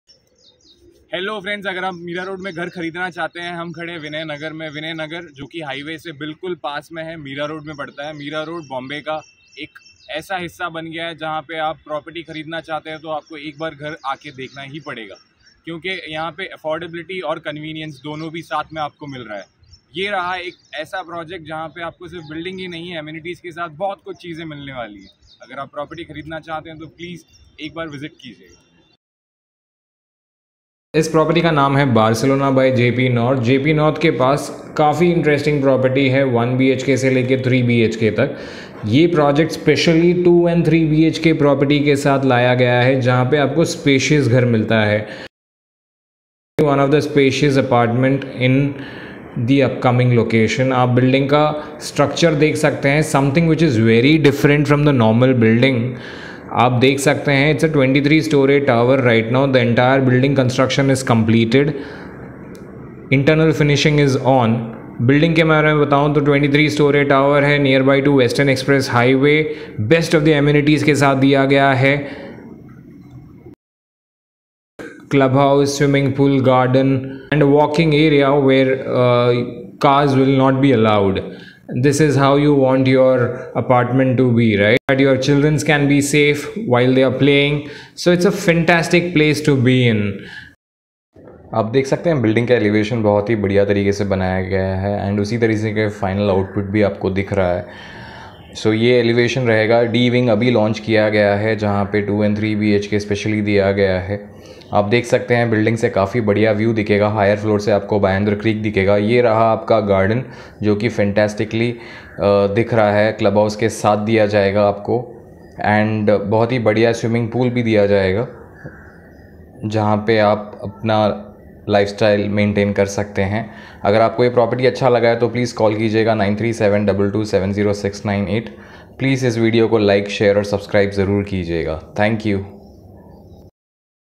Buy Home in Miraroad | 1BHK | 2BHK | 3BHKin Miraroad | Flat for sale
JP North Barcelona | Hot Cake | Garden City | The hottest selling project in Mira Road East, Mumbai
JP INFRA presents an innovation like never seen before. PREMIUM 2 & 3 BHK HOMES WITH DEDICATED WORK FROM HOME SPACES just for you. All your meetings, presentations and family time can now be managed without any hassle.

At North Barcelona, everything is designed to appeal to a wide range of age groups and help each person discover the innocence of simple pleasures, made more grand by sheer attention to detail.